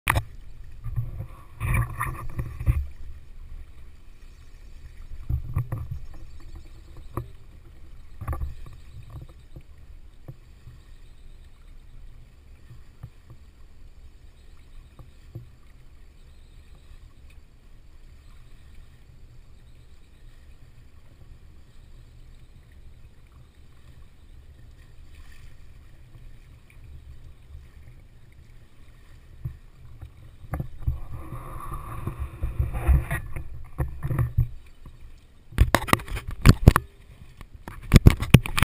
Sailing on the Chesapeake Bay. August 2014. 5-10 kts of wind
Using a GoPro on a boat hook.